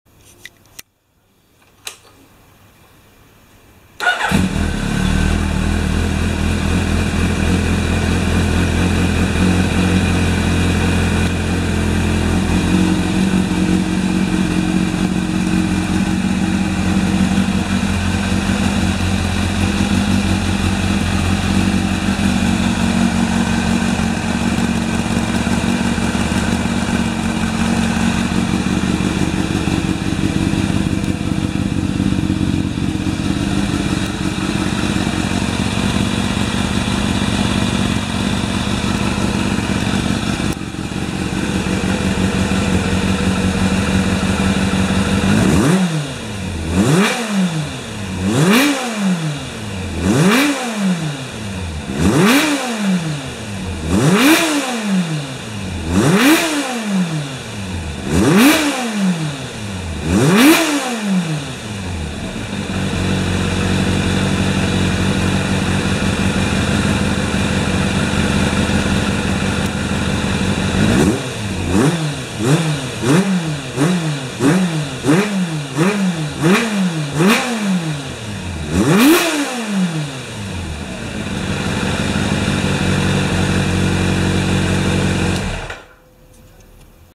GPZ900RにＣＲキャブレター～走行前のプリセット作業
カワサキＧＰＺ９００ＲにＣＲ－ＭＢ３５を装着しています。
コックとキャブレターボディーの干渉がありますので、別のコックを手配することとしました。
走れませんので、とりあえず走行前のプリセッティングまでを行いましたが、まあ、問題はなさそうですね。
なかなかの吹けを見せてくれます。
コックが届いたら実走にて煮詰めを行いましょう。